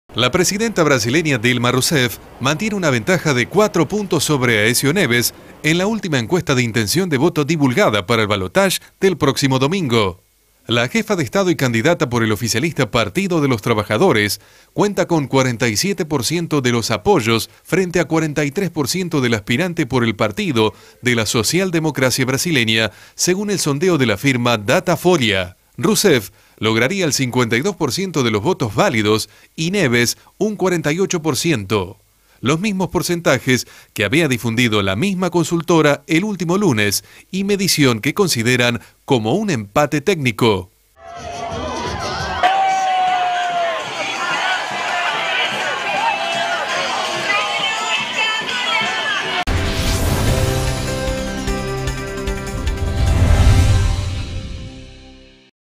Una nueva encuesta da a Rousseff cuatro puntos de ventaja para el balotaje en Brasil
La presidenta brasileña y candidata a la reelección, Dilma Rousseff, mantenía hoy una ventaja de cuatro puntos porcentuales sobre el opositor Aécio Neves en la última encuesta de intención de voto divulgada para el balotaje del próximo domingo, medición que las consultoras consideran como de empate técnico, debido al margen de error de al menos 2% que tienen estas muestras.

La jefa de Estado y candidata por el oficialista Partido de los Trabajadores (PT) cuenta con 47% de los apoyos frente a 43% del aspirante por el opositor Partido de la Social Democracia Brasileña (PSBD), según el sondeo de la firma Datafolha.

El lunes último, tanto esta consultora como Vox Populi habían dado sondeos que arrojaban similar diferencia y mostraban, por primera vez desde el final de la primera vuelta electoral, a Dilma por delante de Aécio.

Según Datafolha, citada por Efe, 6% de los encuestados pretende votar en blanco o anular su sufragio y 4% aún está indeciso.

Considerando los votos válidos, es decir sin tener en cuenta los blancos o nulos como marca la legislación electoral brasileña, Rousseff lograría 52 % de los votos y Neves 48 %, los mismos porcentajes que en la encuesta del lunes.

De acuerdo con los técnicos de Datafolha, la fuerte reacción de la jefe de Estado en las encuestas puede ser explicada por el menor pesimismo de los brasileños con respecto a la economía, ya que 50% de los consultados consideraba en septiembre que la inflación iba a subir y ese porcentaje cayó a 31% en octubre.

El avance de la presidenta en las encuestas también se vio favorecido por un mayor apoyo entre las electoras, pues el porcentaje de mujeres dispuesto a votar a Rousseff creció desde 42% el 9 de octubre a 47 % en la encuesta de ayer.

En la primera vuelta de las presidenciales, Rousseff se impuso con el 41,5% de los votos a Neves, que logró 33,5 %.